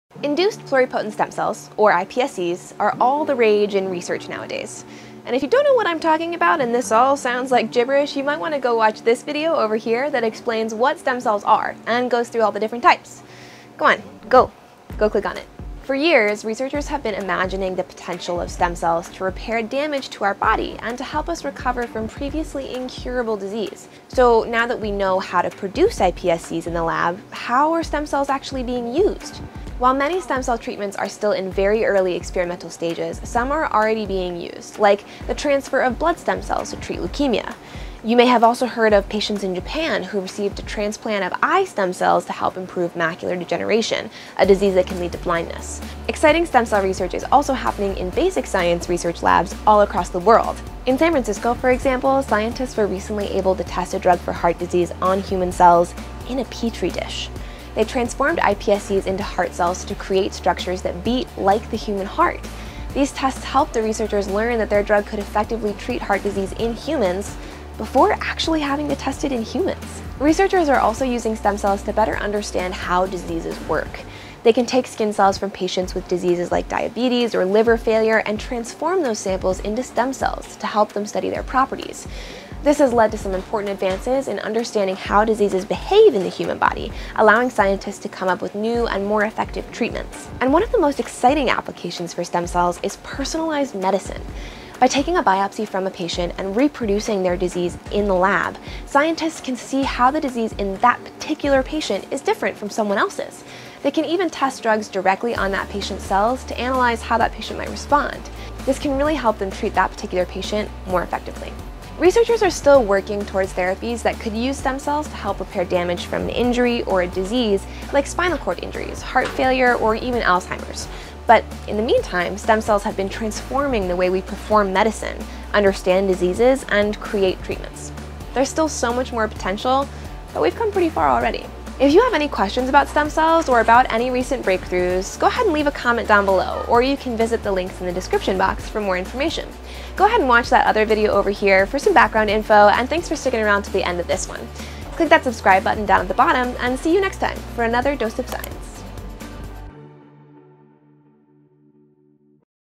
How Are Stem Cells Being Used in Research? | Dose of Science
Stem cells can now be produced in the lab, but how are they being used? Do they really have the potential to transform medicine? In this Dose of Science, find out what's happening in stem cell research today!